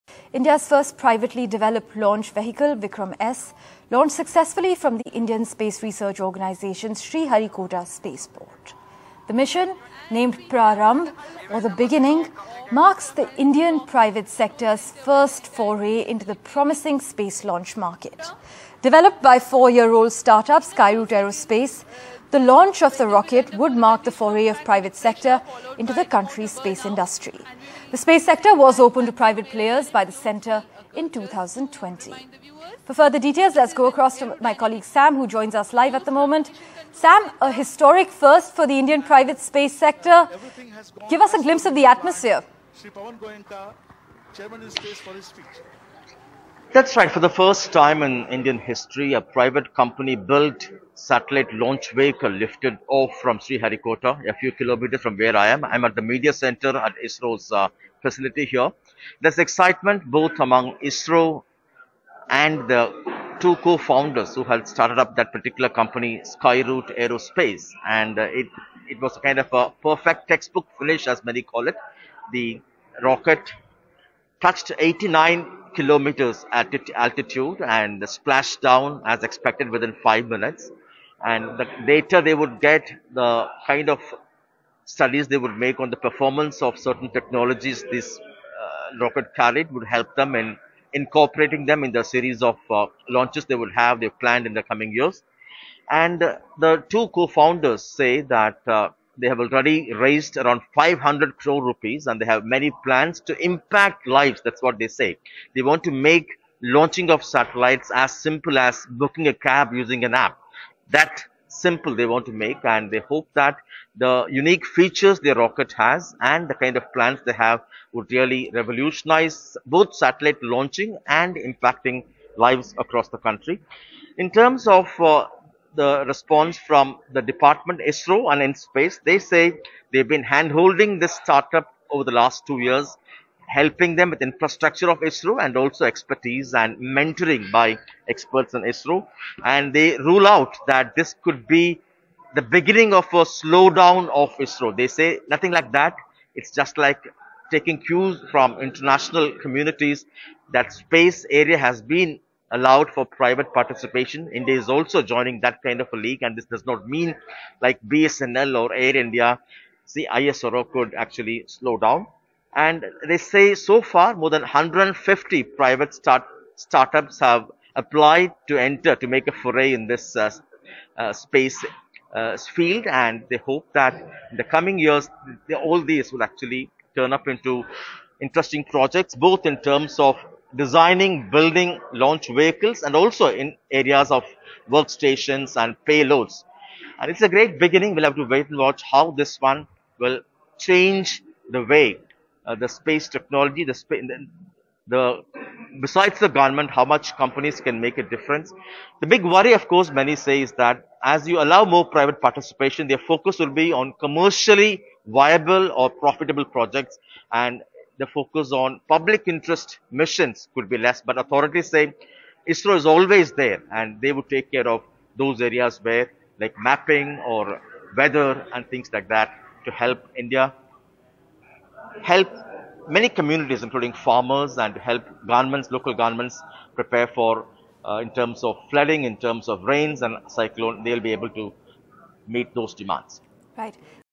India's First Privately-Built Rocket, Vikram-S, Launched
Vikram-S Launch: Vikram-S, India's first privately developed rocket, was successfully launched by the Indian Space Research Organisation (ISRO) from its spaceport in Sriharikota, about 115 kms from Chennai, today.